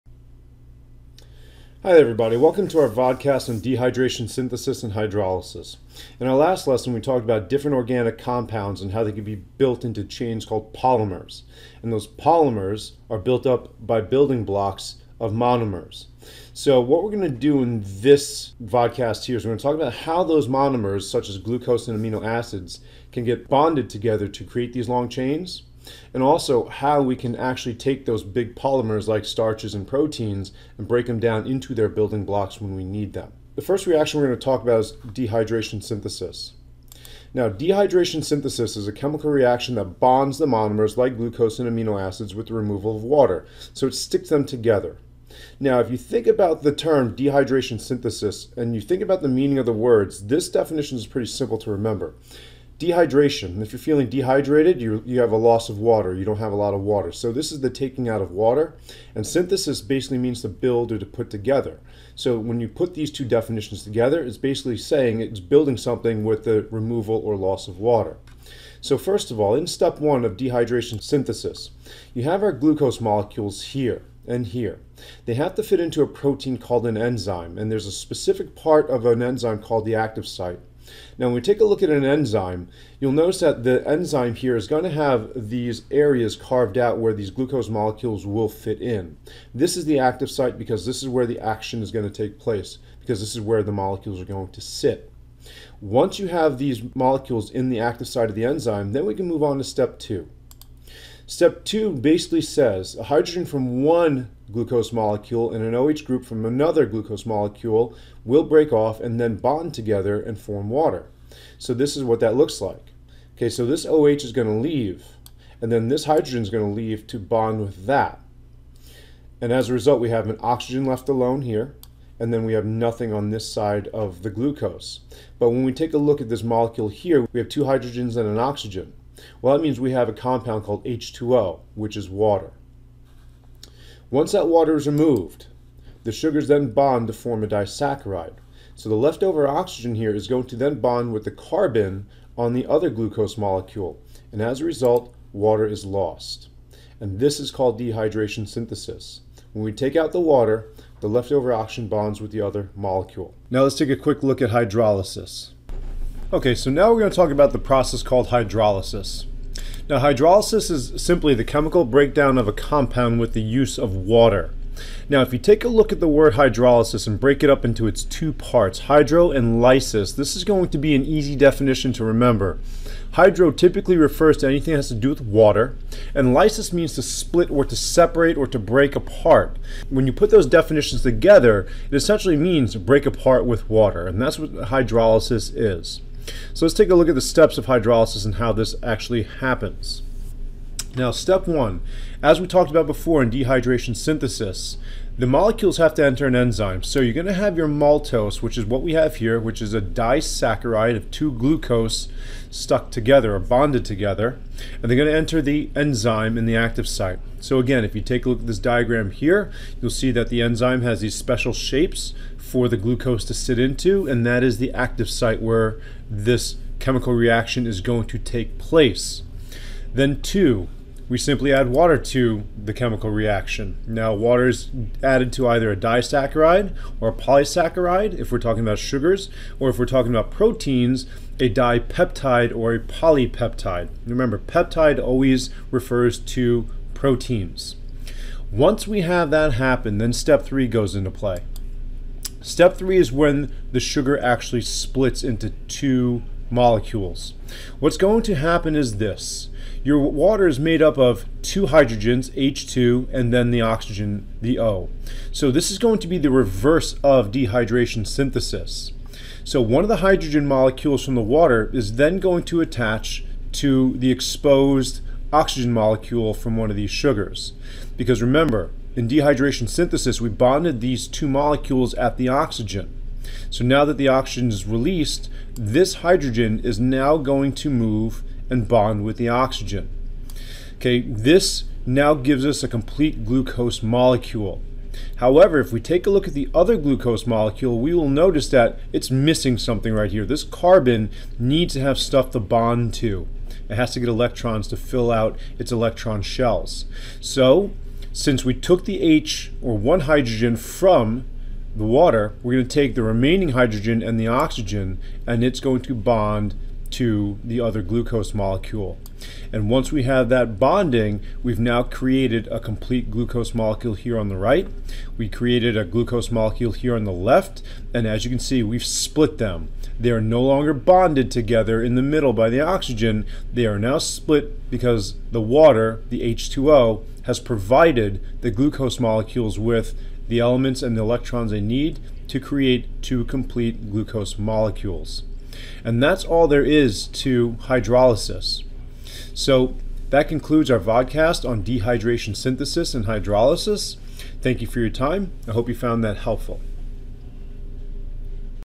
Dehydration Synthesis and Hydrolysis
This vodcast goes over how dehydration synthesis and hydrolysis works when bonding or breaking organic polymers